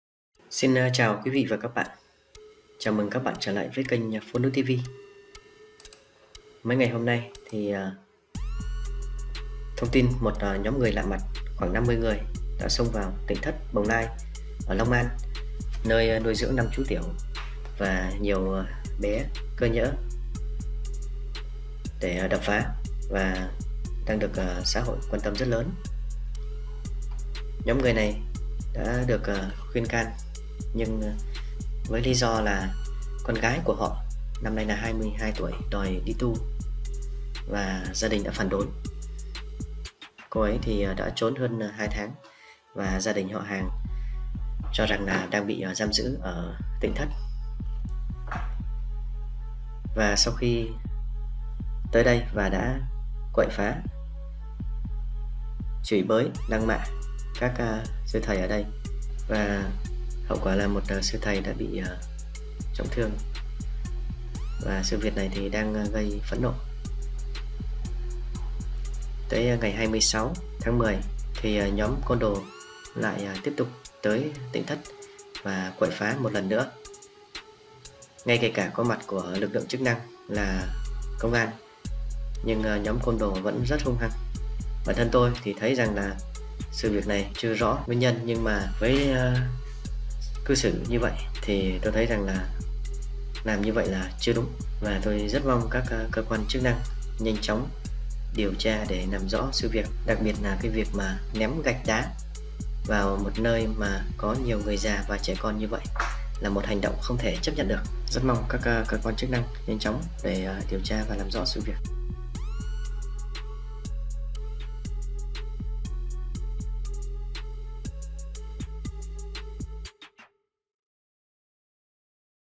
Cần lên án "nhóm côn đồ" Đ.Ậ.P P.H.Á Tịnh thất BỒNG LAI|PhoNui TV
Rất mong các cơ quan chức năng cần điều tra làm rõ "nhóm côn đồ" Đ.Ậ.P P.H.Á  tịnh thất BỒNG LAI để trả lại công bằng cho Tịnh Thất Bồng Lai và bảo vệ cho các chú tiểu cũng như mọi ngời ở đây.